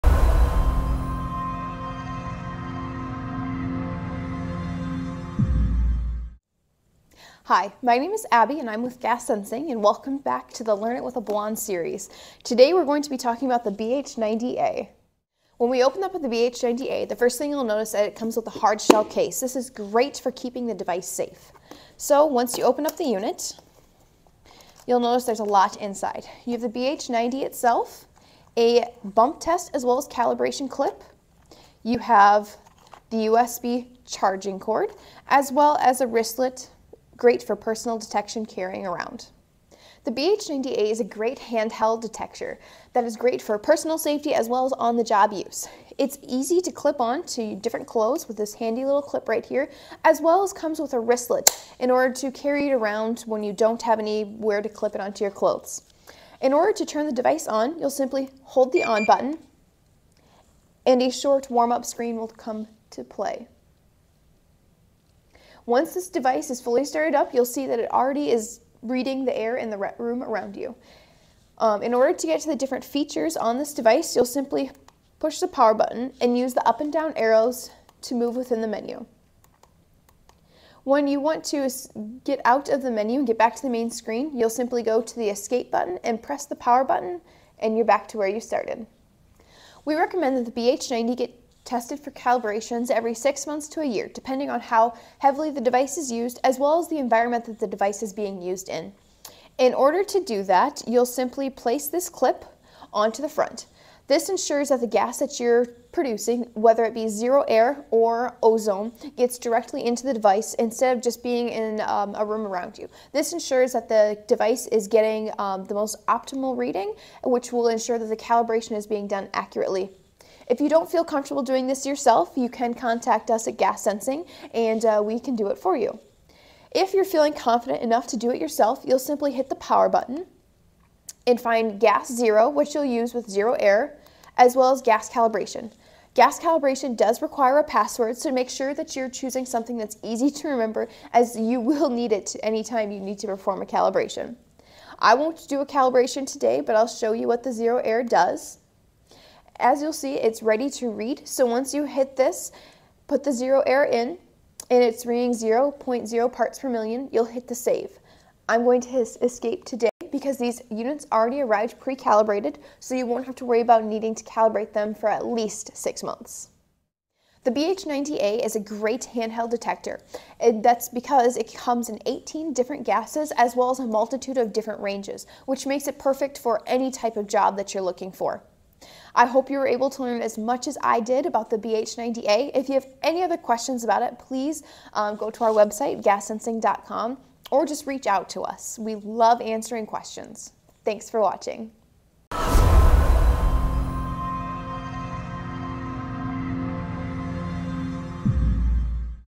Learn it with a Blonde: How to use the BH-90A Handheld Gas Detector
The BH-90A gas detector is a cutting-edge product that is revolutionizing the way we detect gas leaks. This portable device is designed to detect a wide range of gases, including methane, propane, hydrogen, carbon monoxide, and many others.

The BH-90A is incredibly easy to use. Simply turn on the device, and it will start to detect any gas leaks in the area. The detector is equipped with a high-quality sensor that is capable of detecting even the smallest gas leaks, making it a valuable tool for anyone who works in the oil and gas industry, HVAC technicians, firefighters, and more.

One of the greatest benefits of the BH-90A gas detector is its portability. The device is small and lightweight, making it easy to carry with you wherever you go. This makes it ideal for use in remote areas, confined spaces, or for emergency response situations.

The BH-90A gas detector is also incredibly durable. It is made of high-quality materials that are designed to withstand tough conditions. The device is also equipped with a rechargeable battery, which means you can use it for extended periods without needing to replace the battery.

Another benefit of the BH-90A gas detector is its accuracy. The device is capable of detecting gas leaks in real-time, giving you the ability to take immediate action to prevent potential hazards. The detector is also equipped with an alarm system that will alert you if it detects a gas leak, giving you the opportunity to evacuate the area quickly and safely.

The BH-90A gas detector is an excellent investment for anyone who works in the oil and gas industry, HVAC technicians, firefighters, and more. It is portable, easy to use, and incredibly accurate, making it a valuable tool for detecting gas leaks and preventing potential hazards. With its high-quality sensor and durable design, the BH-90A gas detector is a must-have tool for anyone who values safety and efficiency in their work.

You can find the BH-90A on our website!